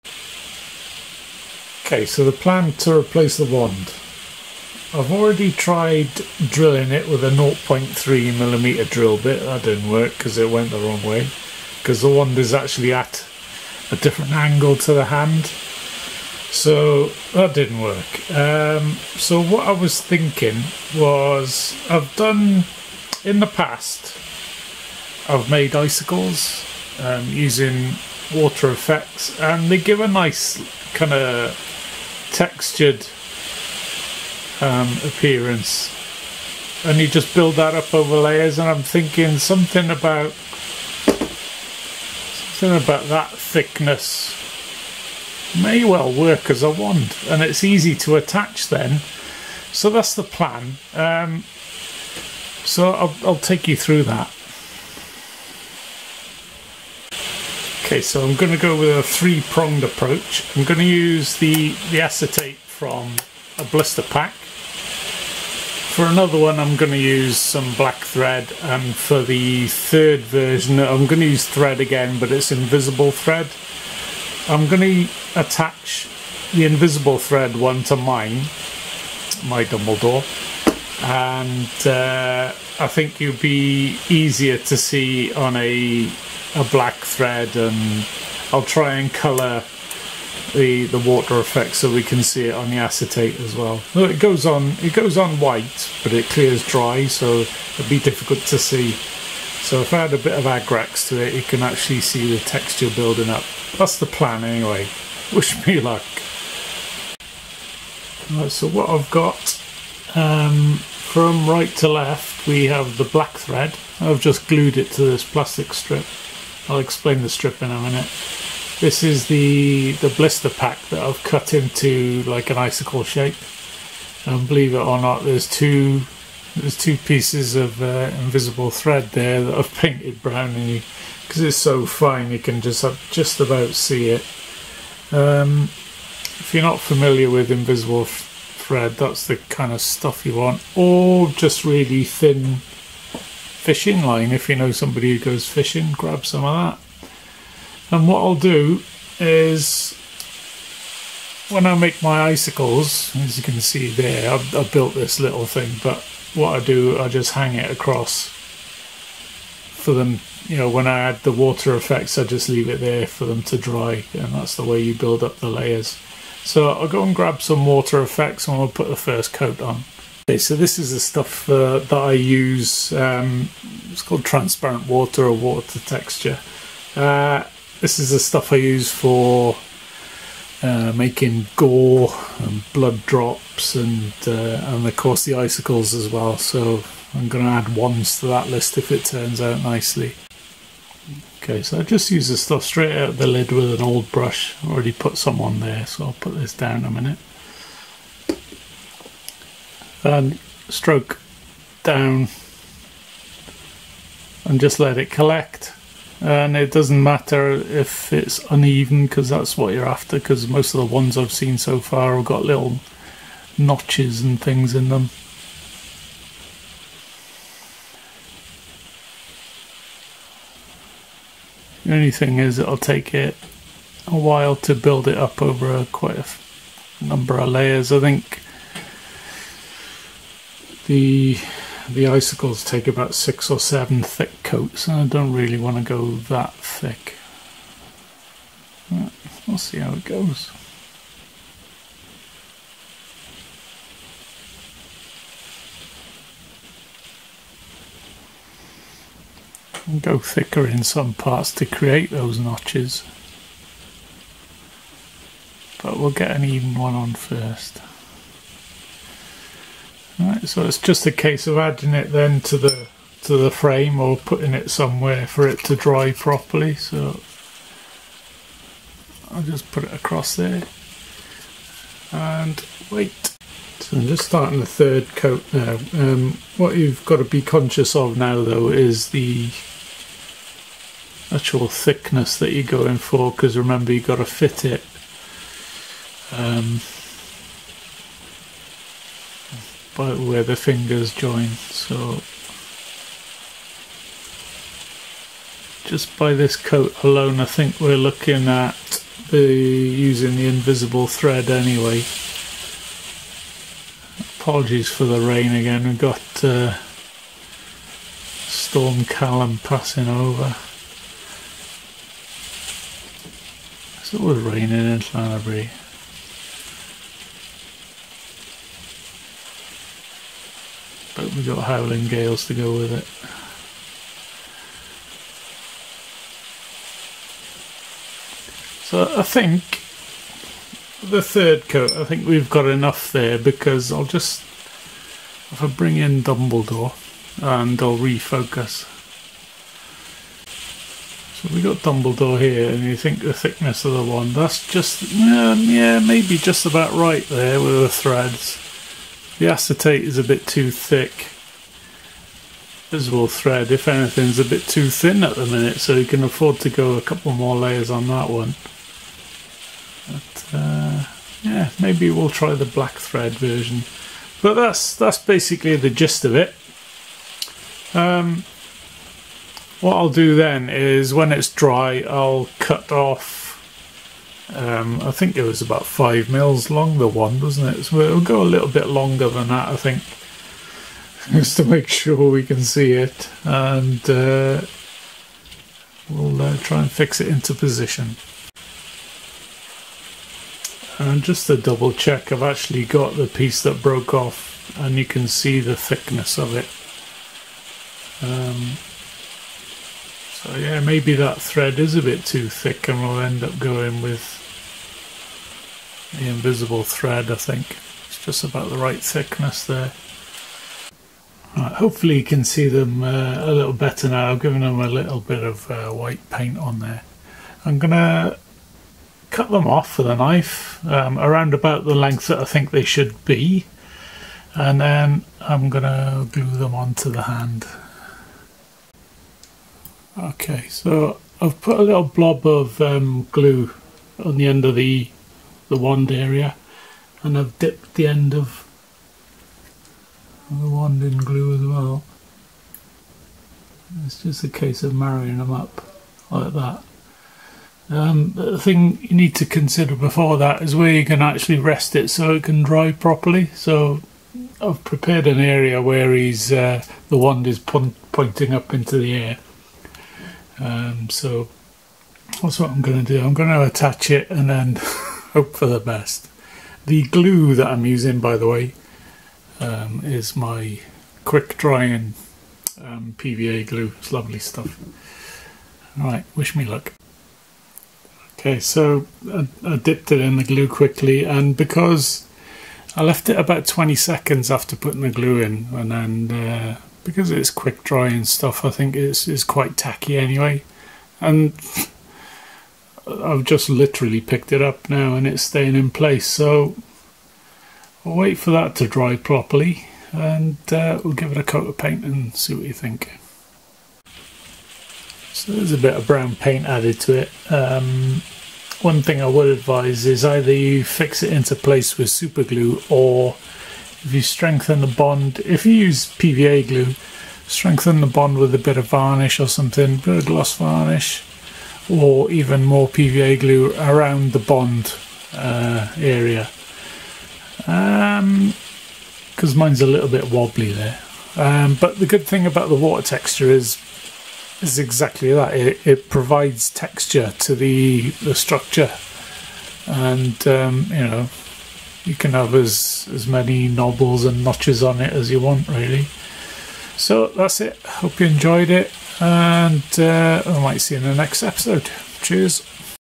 Painting miniatures tutorial for the Harry Potter Miniature Gamel - Repairing Wands
Painting miniatures tutorial for the Harry Potter Miniature Game
Here's a quick tutorial on how I mended a broken wand for Dumbledore from the HPMAG by Knight Models.

You will need the following:
Craft knife or scalpel
Invisible thread or thin fishing line
Vallejo Water Texture or something similar
Fast drying PVA glue or superglue
A bit of patience :-)